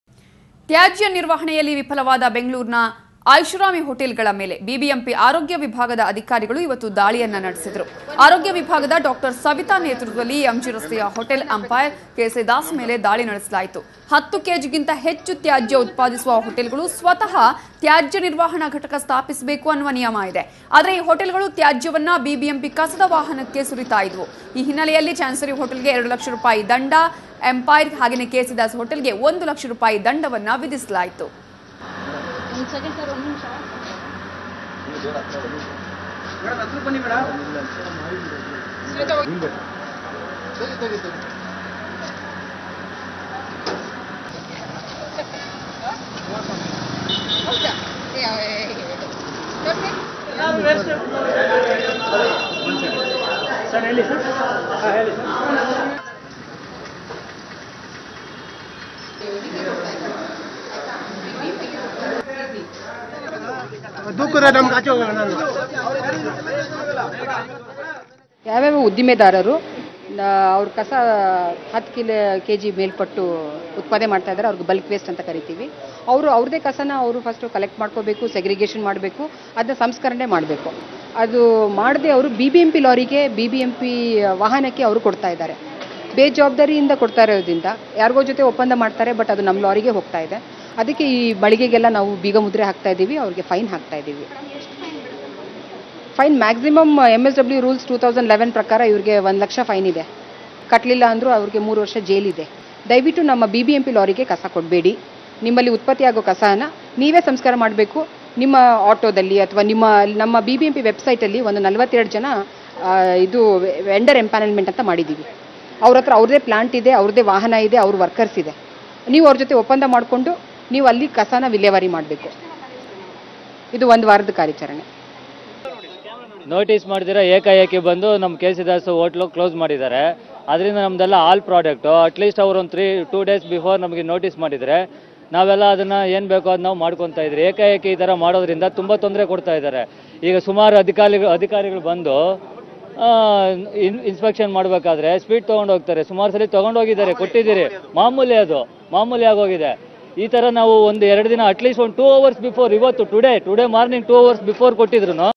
Big hotels fined for garbage disposal failure
Suvarna News 24X7 - 30 Apr 15 , Big hotels fined for garbage disposal failure